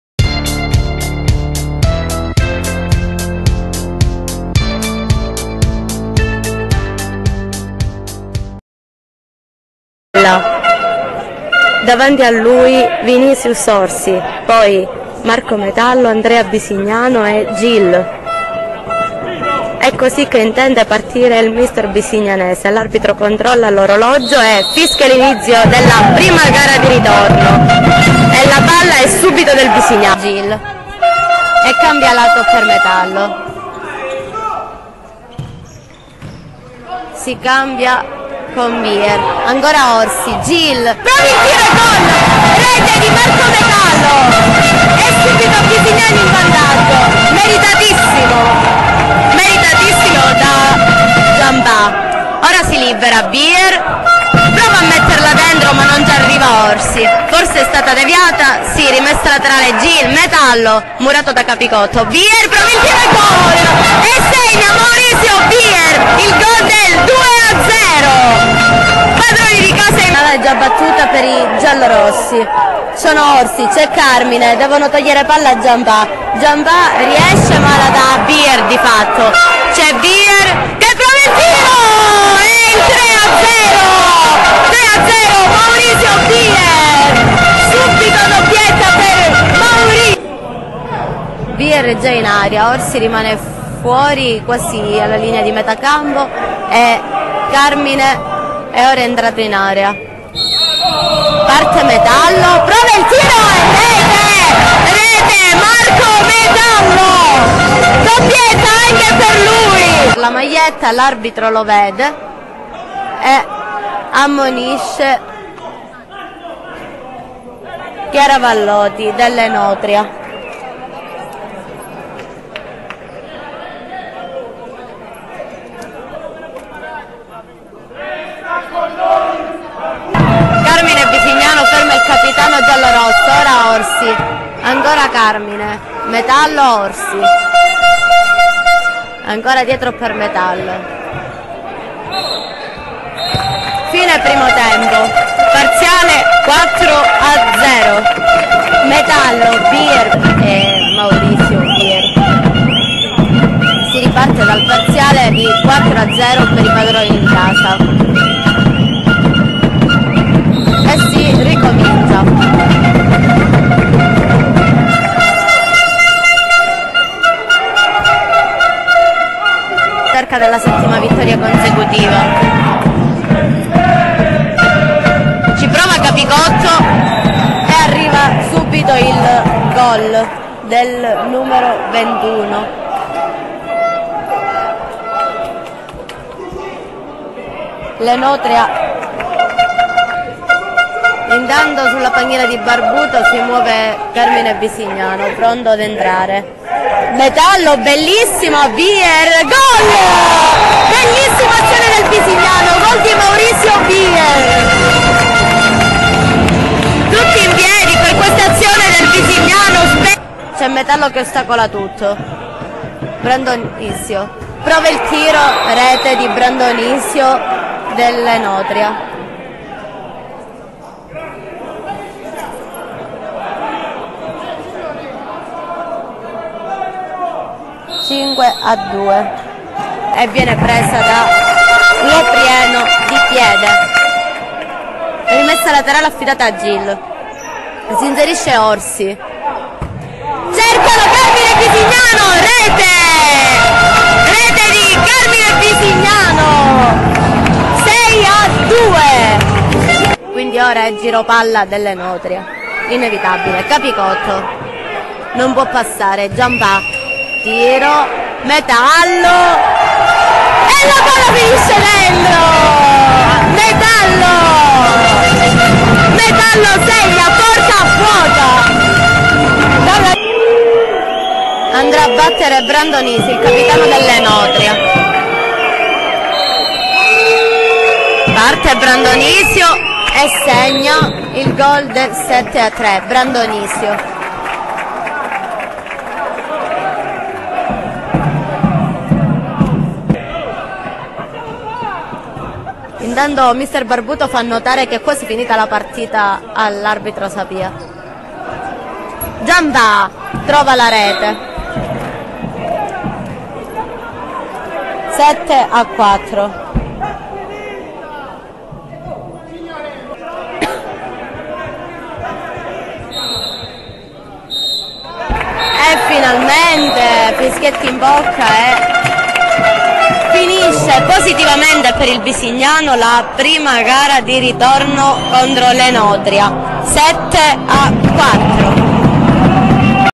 Highlights Bisignano Futsal Magic Games - ASD Enotria Catanzaro (Finale 7-4)
Quattordicesima giornata (prima di Ritorno) del massimo campionato Regionale di Futsal. Finisce 7-4 per il Bisignano contro l'ASD Enotria di Catanzaro. Goal e spettacolo !!